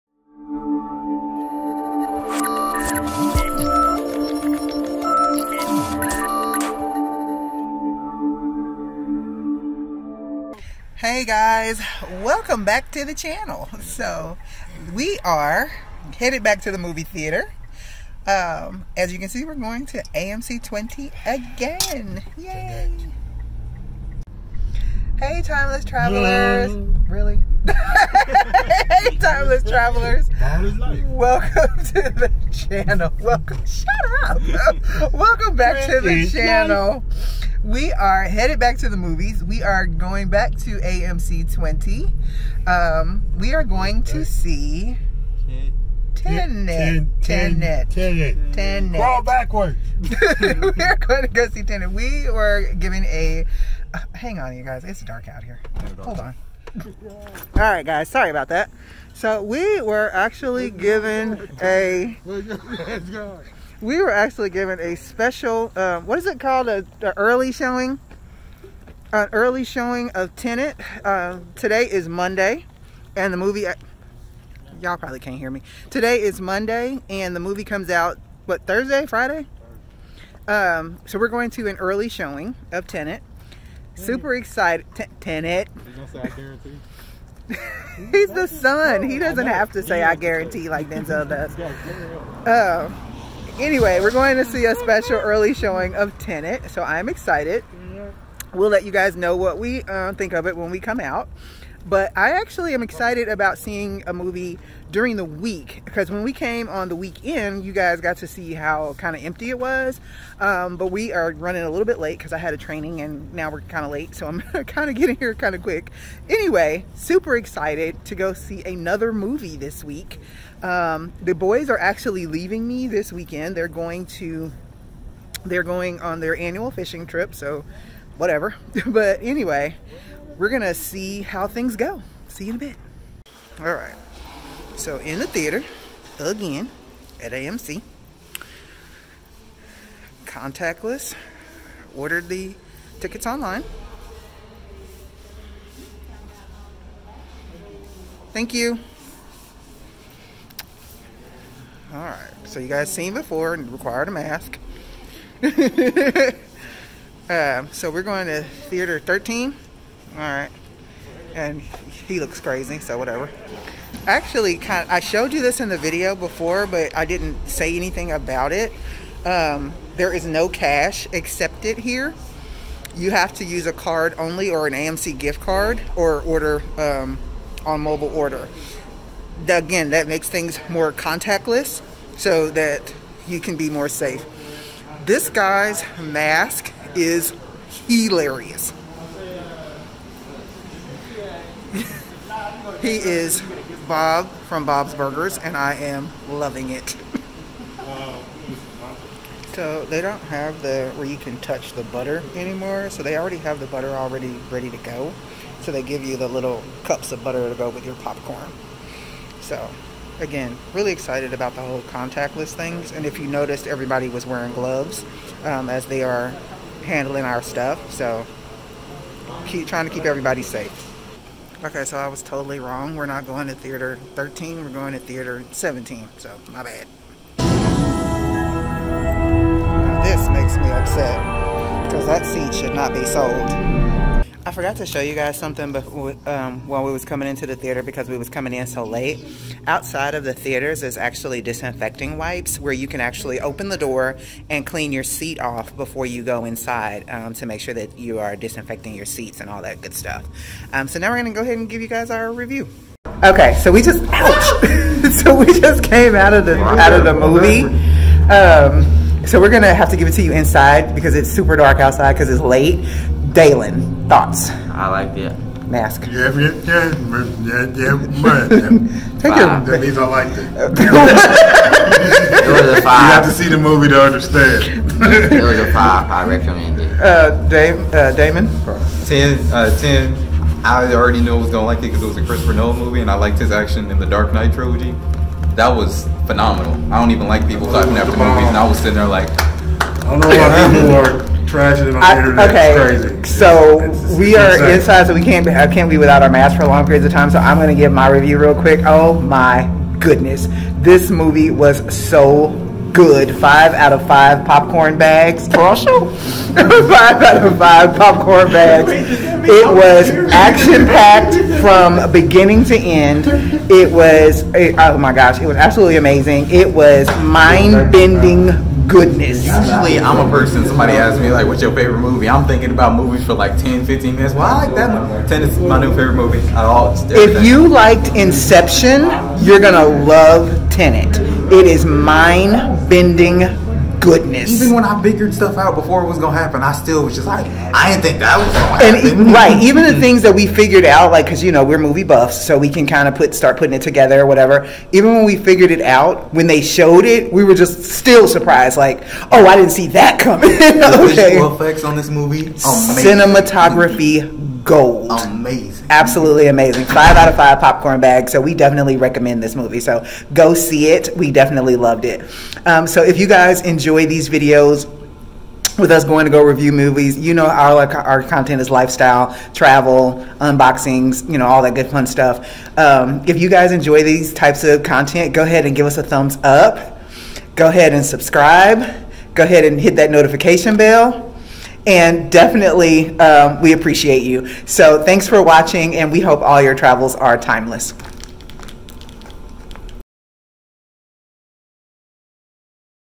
Back to the Movies! - Tenet Movie Review
Trish, Will, Damon and Dalyn head back to the newly reopened AMC movie theater in Kansas City to an early showing of the Christopher Nolan movie, Tenet. We give you our review of the movie as well as some more information about the safety practices of the theater.